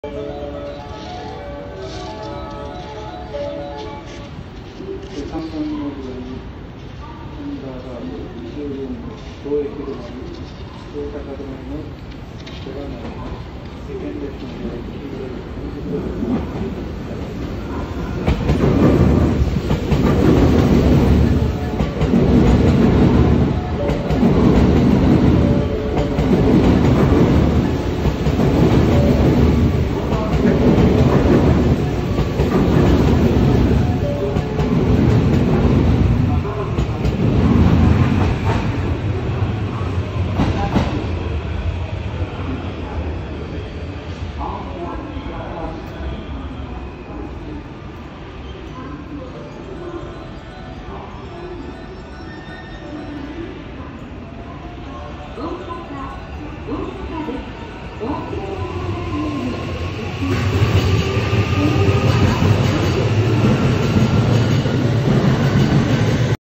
JR西日本 683系4000番台 特急 【サンダーバード】 大阪行
東海道本線　大阪駅3.4番線ホームにて撮影。
リニューアル車9両編成です。
大阪止まりの列車です。